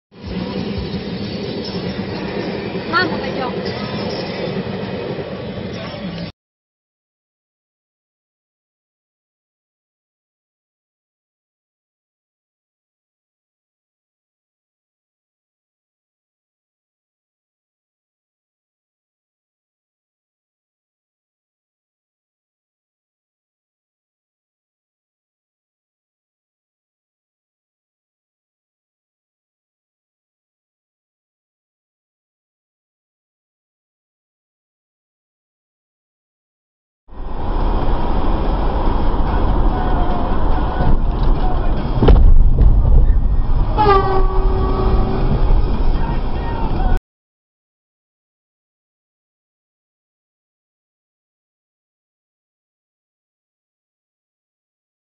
Epopej slovenských ciest (7.)
Zmes videí od vás. Pamätajte, ak máte na autokamere zachytený dopravný priestupok, zoberte ho a choďte na políciu - vaša výpoveď je nevyhnutná.